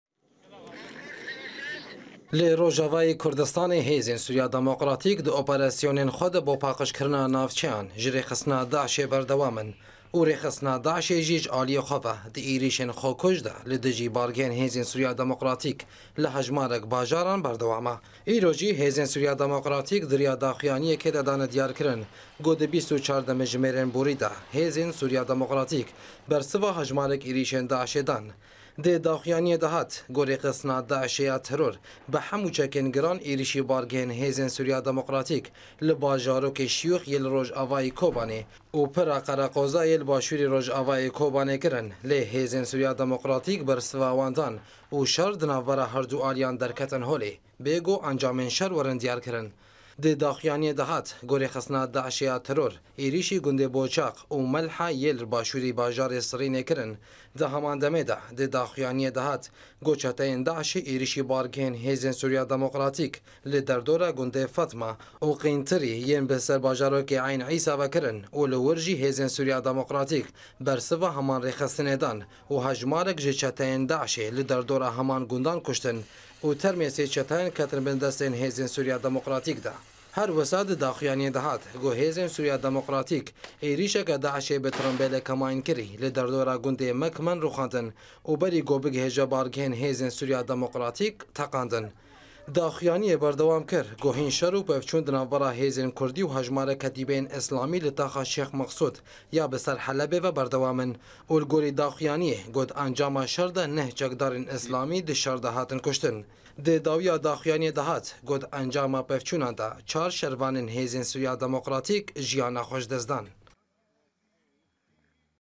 Hêzên demoqratîk êrîşên DAIŞê li Kobanî rûxandin
Di 24 demjimêrên bûrî de rêxistina DAIŞê êrîşî hejmarek bargehên hêzên
Sûriya demoqratîk li hejmarek bajaran kirin û hêzên demoqratîk jî ji aliyê
xwe ve di berevaniyê de berdewame